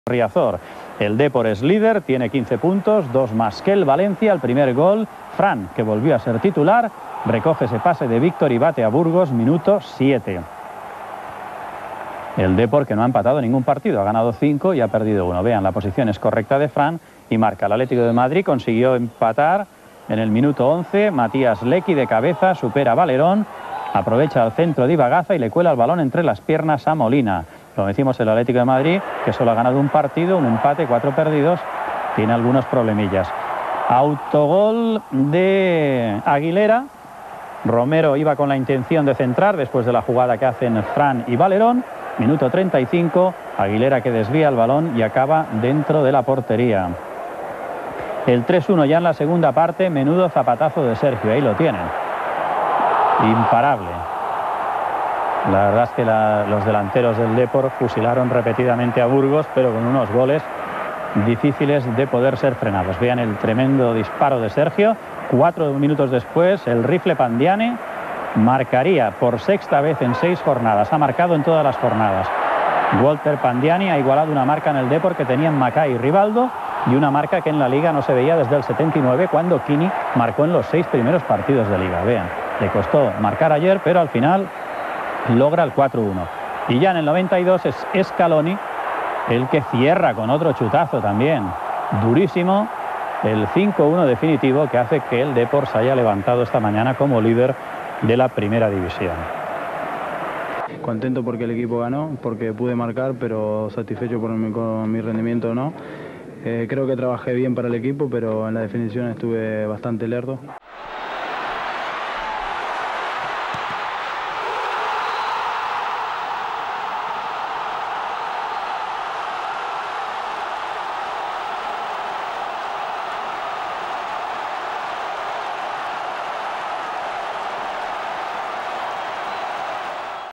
Deportivo 5-1 Atlético de Madrid | Resumen | Liga 03/04 | Jornada 6
Gran goleada del Deportivo ante el Atleti de Fernando Torres. Resumen del partido, y declaraciones del delantero uruguayo Walter Pandiani tras el choque.